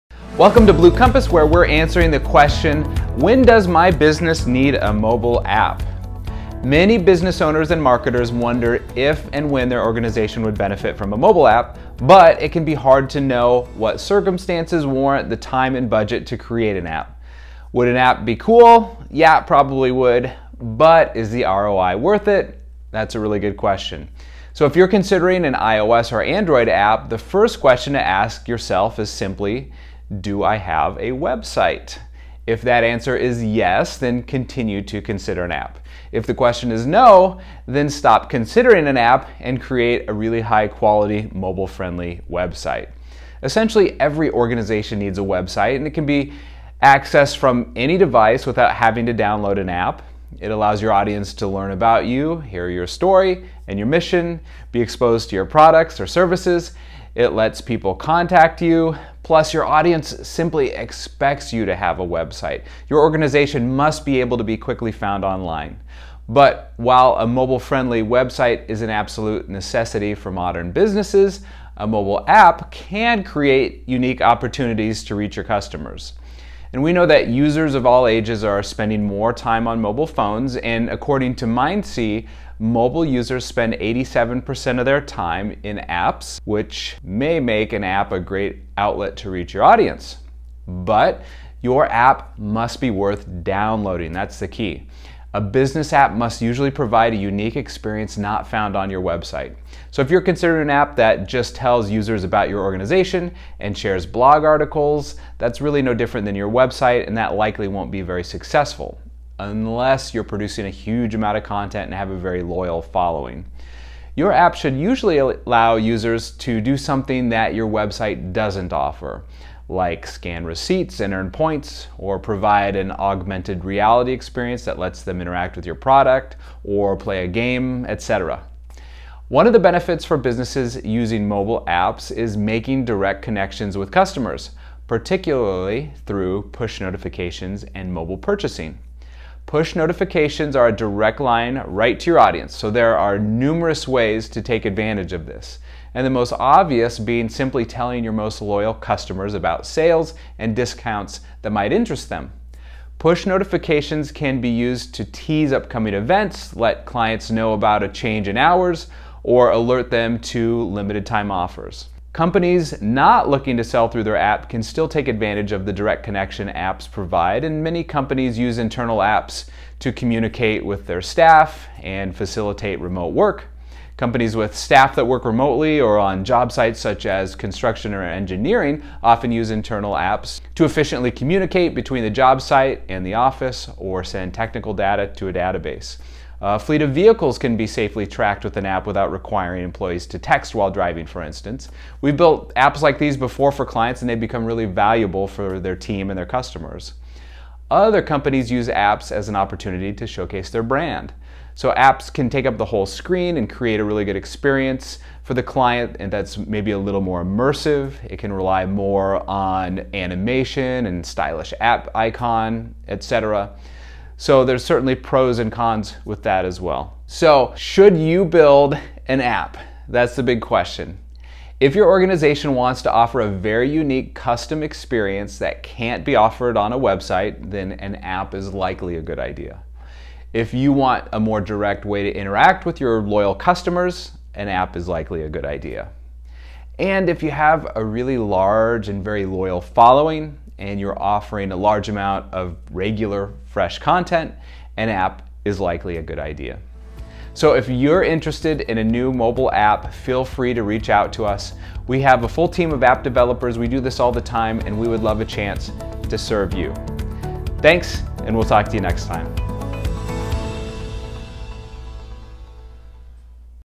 Does Your Business Need a Mobile App?
Discover three signs that your organization will benefit from creating a mobile app.

Learn about the differences between websites and mobile apps and when your organization needs to move beyond just a website to an app. Blue Compass President & Co-founder, Drew Harden, shares why how apps can be beneficial for creating unique experiences and making deeper connections with customers.

TIMESTAMPS
0:00 Intro
0:24 When is a mobile app needed over a website?
1:15 The potential audience for mobile apps
1:30 Why your mobile app must be worth downloading
2:09 The advantages of push notifications for businesses
2:47 Advantages of apps for companies internal use
3:17 Mobile apps can be more immersive than websites
3:36 Three signs your business should consider a mobile app
4:07 Closing